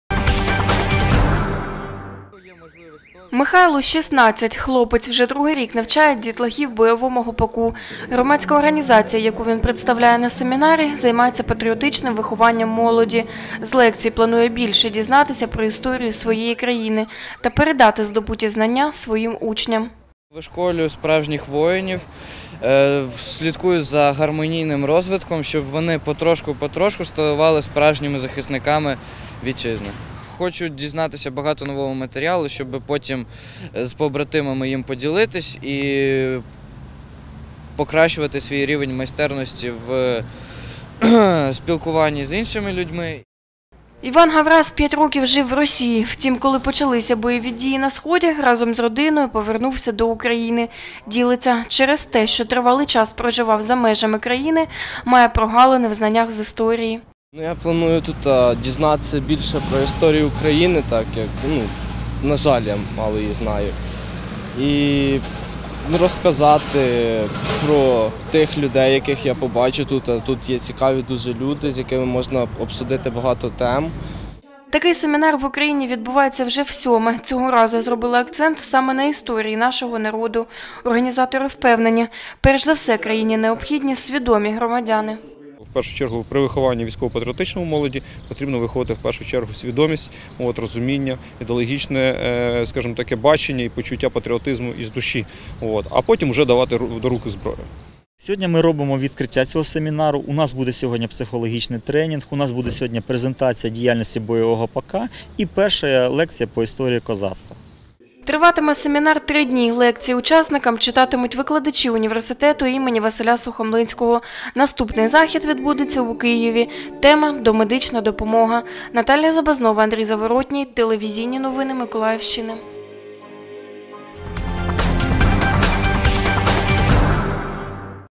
У Миколаєві стартував всеукраїнський семінар “Кузня захисників України”
Понад 70 учасників та близько десятка лекцій з  історії України. У Миколаєві стартував всеукраїнський семінар “Кузня захисників України”.